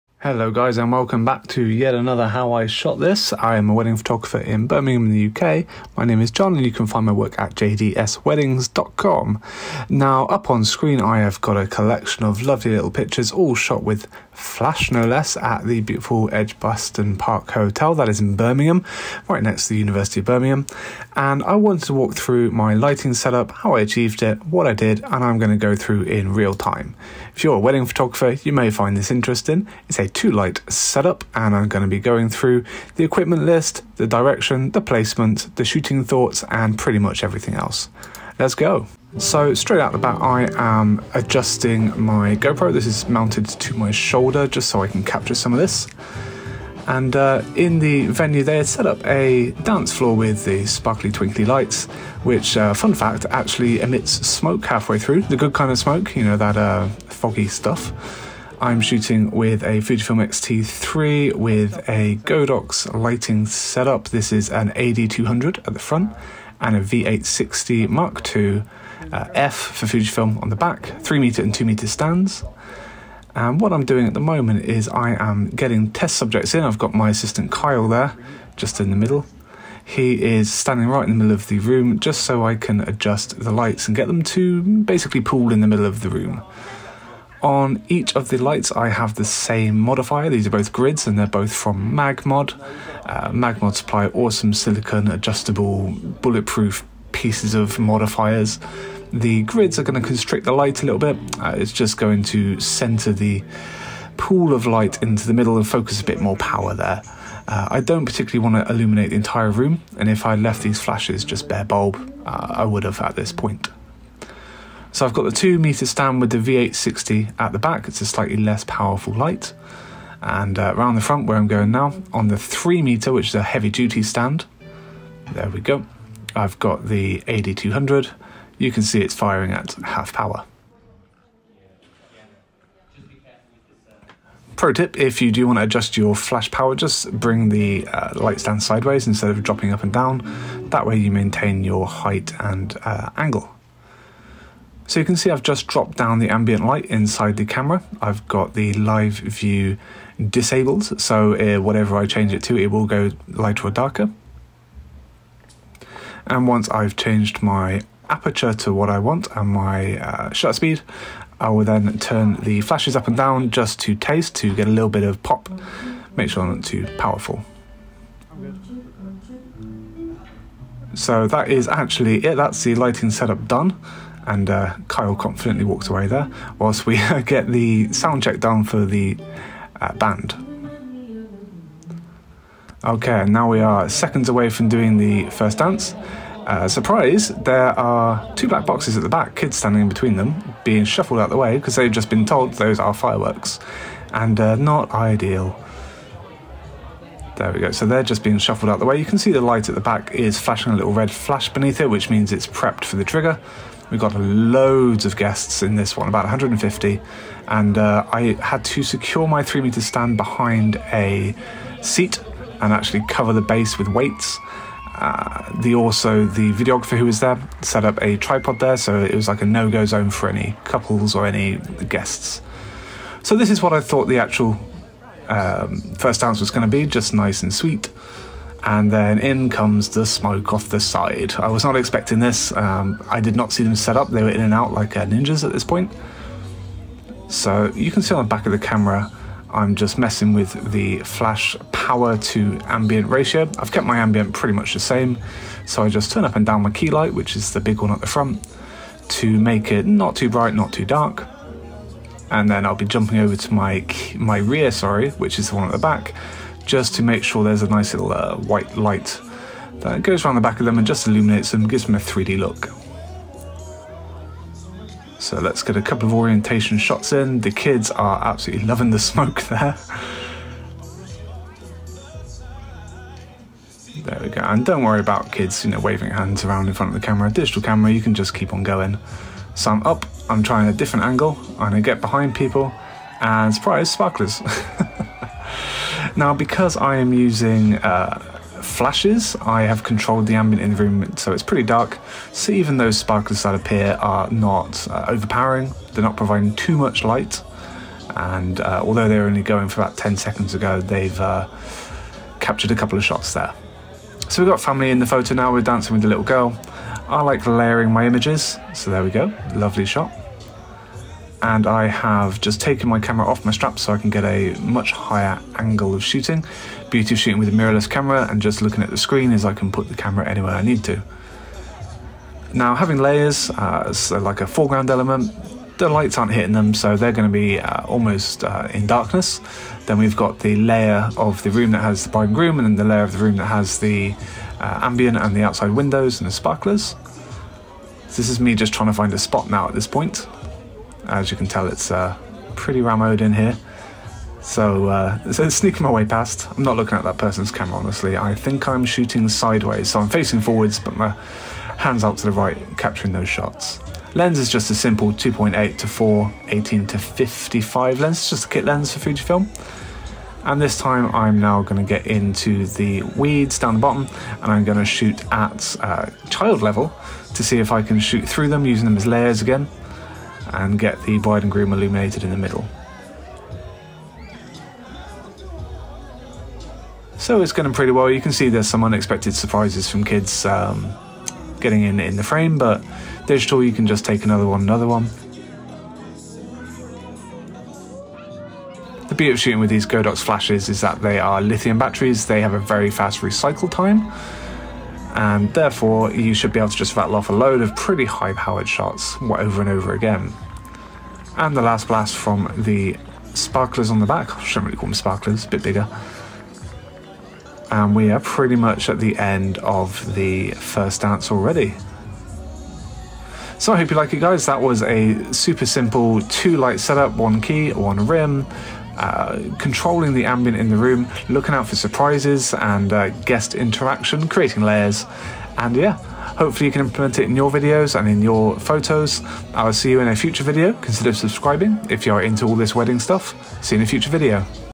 How I Shot It - First Dance Lighting Setup - Fujifilm Wedding Photography
Using a Fujifilm X-T3, Godox Ad200 and Godox V860ii-F, I walk you through how I set up my lighting for a first dance at a wedding.

By popular request (nearly) every photo from the setup and shooting process is included, unedited.

Edited with Mastin Labs Portra Pushed presets.